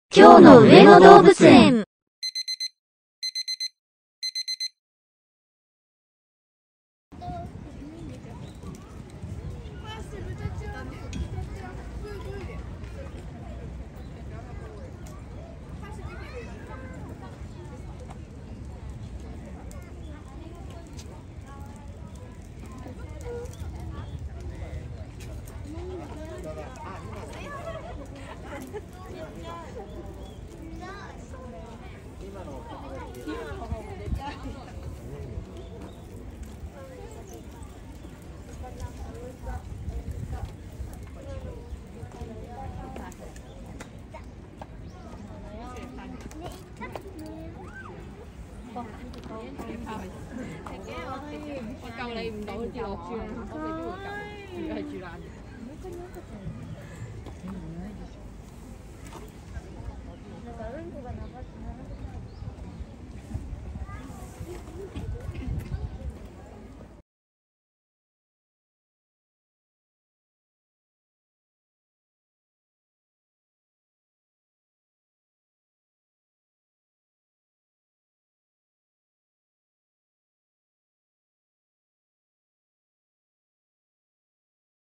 ナビィちゃん、カマデーちゃん あけましておめでとう【2023年1月5日】【上野動物園】アグー、アヨー
【2023年1月5日】【上野動物園】アグー、アヨーの様子です